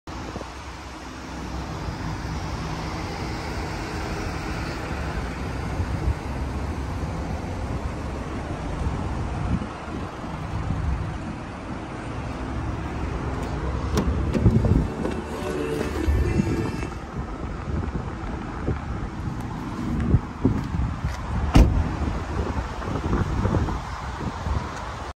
2008 Honda Civic Sdn 4dr Auto LX | Raleigh, NC - Belmonte Auto Imports
Check out the car at belmonteauto.com
Stock #: N210941A
Interior: Leather
Color: Gray
Drive: FWD
VIN: 2HGFA16518H337141
Engine: 1.8L SOHC MPFI 16-valve i-VTEC I4 engine
Trans: Automatic
Mileage: 175,544